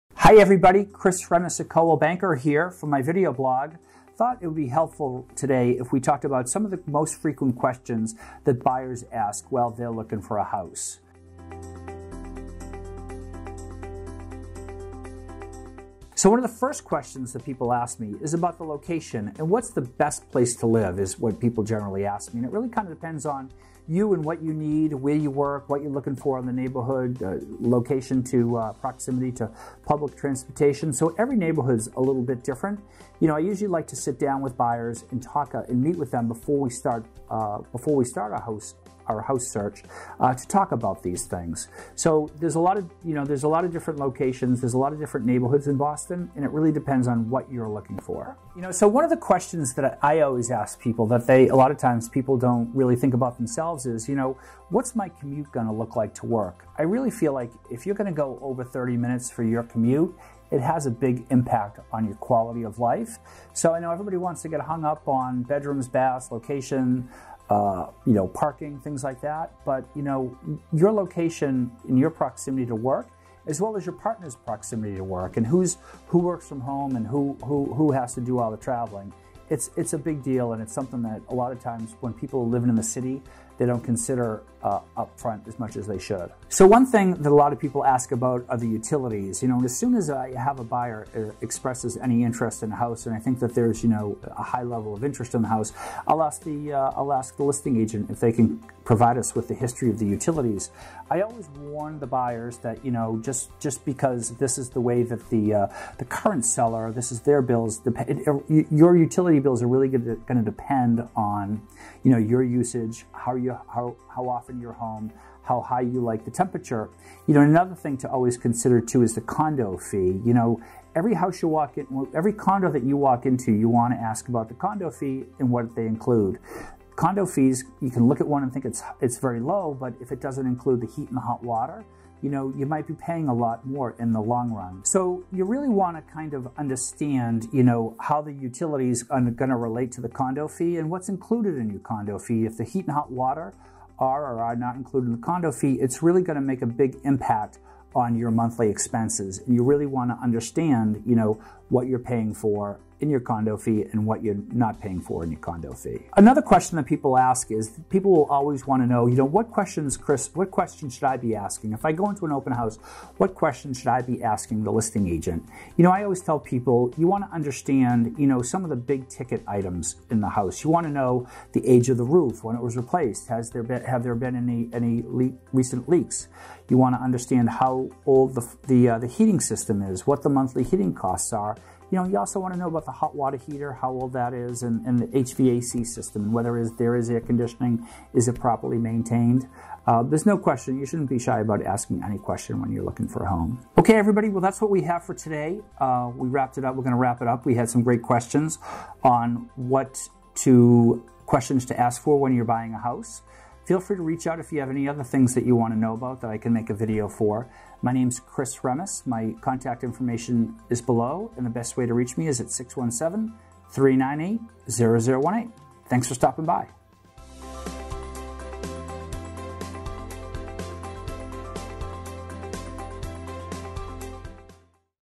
Boston Real Estate - What to Ask During Your House Hunt - Chris Remmes [2021]
Hi Everyone!!
Chris Remmes from the cold banker in Boston. Welcome back to the latest version of my video series. Where I try to help you understand and navigate the ins and outs of the Boston real estate market.

Are you on the hunt for the perfect home? If so, there are a few key questions you should ask during the process to ensure the property you set your sights on is as good a match as it seems. First, you may want to ask yourself about what kind of area you’re looking to live in. Are there certain amenities you’d like to live near? Do you want to live on the edge of town or in the middle of the action? To learn more, watch this short video.

✦✦✦ RECOMMENDED VIDEOS ✦✦✦

Chris Remmes
Coldwell Banker Realty
519 Tremont Street
Boston, MA 02116